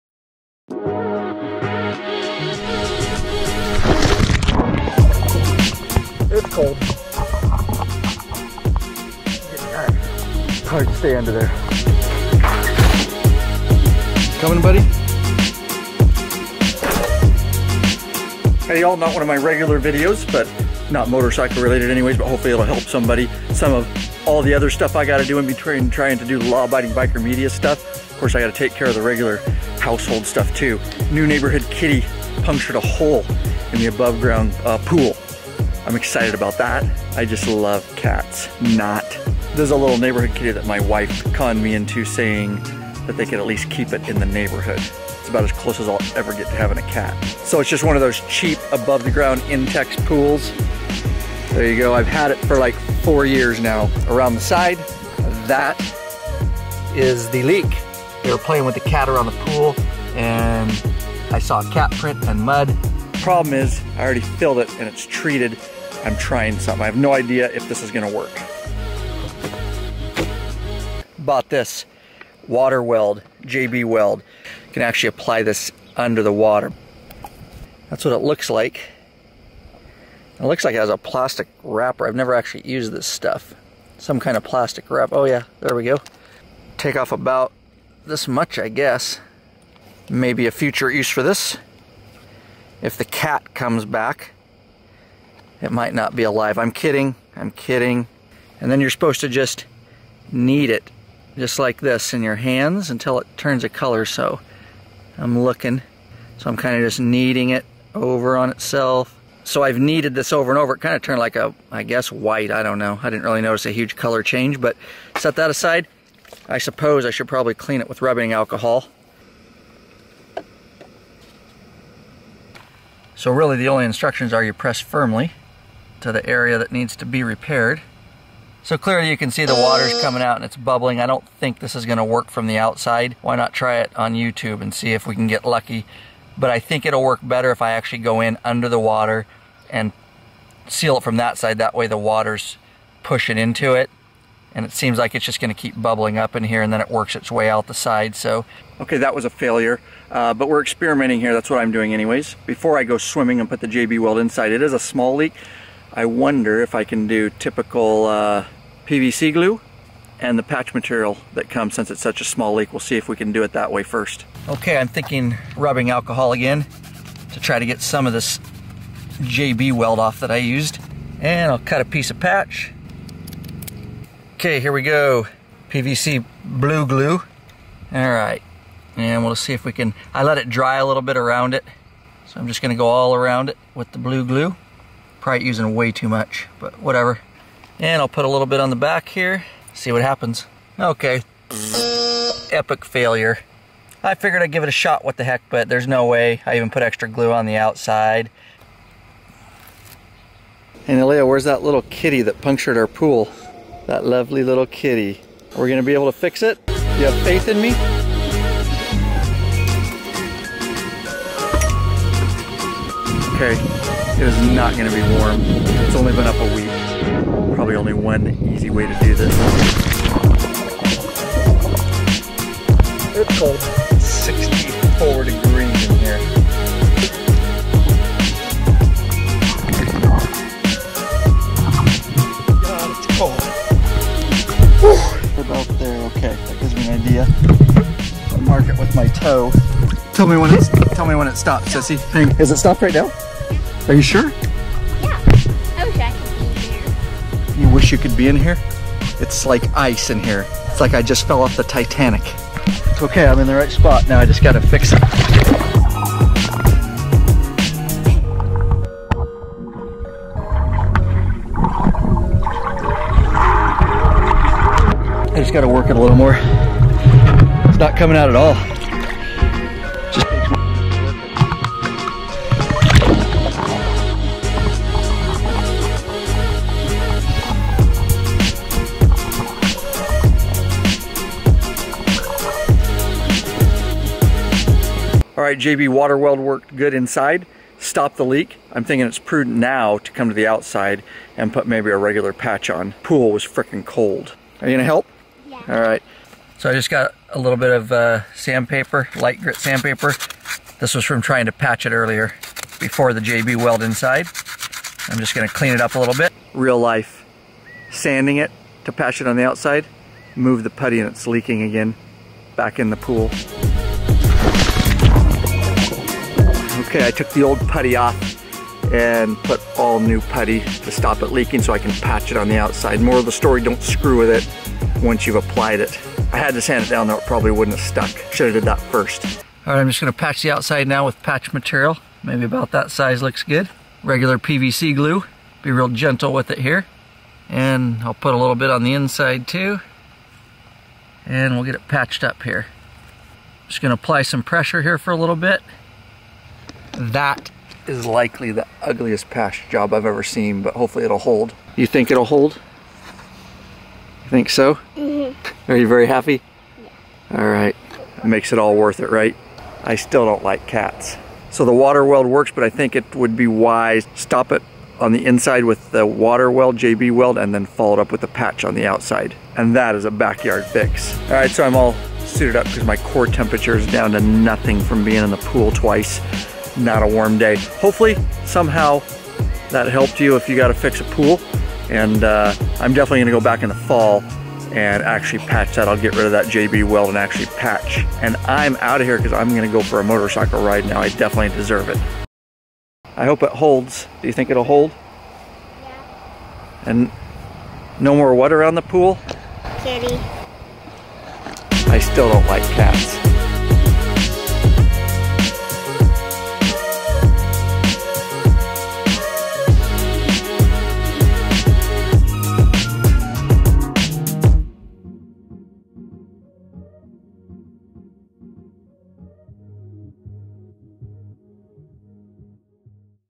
3 Ways to Fix-Repair-Patch Intex Above Ground Pool With Leak-Filled-VLOG
The neighborhood kitten put a hole in the side of our Intex above ground pool liner. The kids thought it'd be a good idea to play near the pool.  No, I don't like cats!  This is not a normal motorcycle related Vlog like I usually do, but just a behind the scenes look and my repair attempts on the pool liner. No, I'm not an expert on pool repair. LOL. I hope it helps someone else out there.

The claw of the kitten put a pin sized hole in the Intex pool liner. I did not want to drain the pool to put on a traditional patch as it had fresh treated water.  I need something that could fix it with the water still in it.

I tried 3 different ways to fix the pool liner. I bought some JB Water Weld. First I tried applying it to the outside and then the inside. The Water Weld can be applied under water, which I show.

After swimming in the frigid pool water two times, I was able to repair the hole in the pool liner with the Water Weld on the inside. Once I stopped the leak, I was then able t o apply a traditional pool liner patch on the outside with PVC glue and patch material.

I will go back this fall when the pool is drained and apply a traditional pool liner patch to the inside and replace the Water Weld.

All is well, and the Intex Pool Liner is fixed and not leaking! No, I still don't like cats and they're not allowed around the pool. Those are the rules now!